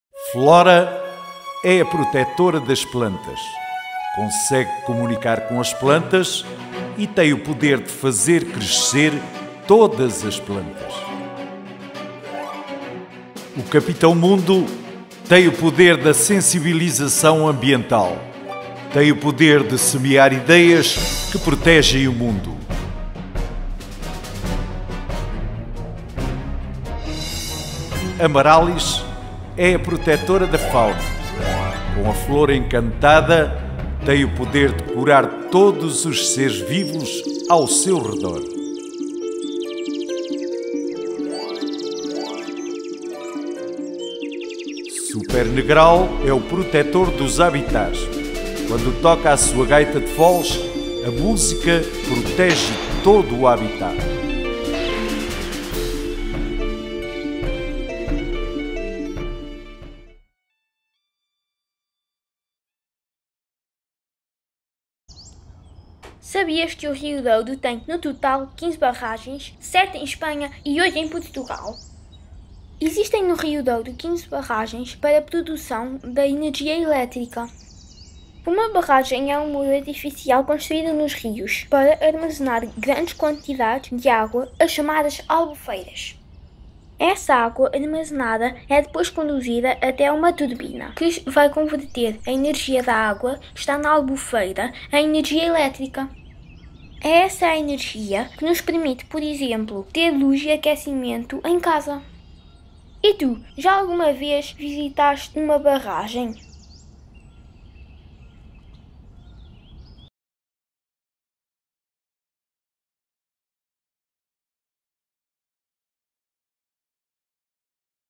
FNT04E03 - PN do Douro Internacional: Barragens no Rio Douro
Sabias que o rio Douro tem, no total, 15 barragens (7 em Espanha e 8 em Portugal)?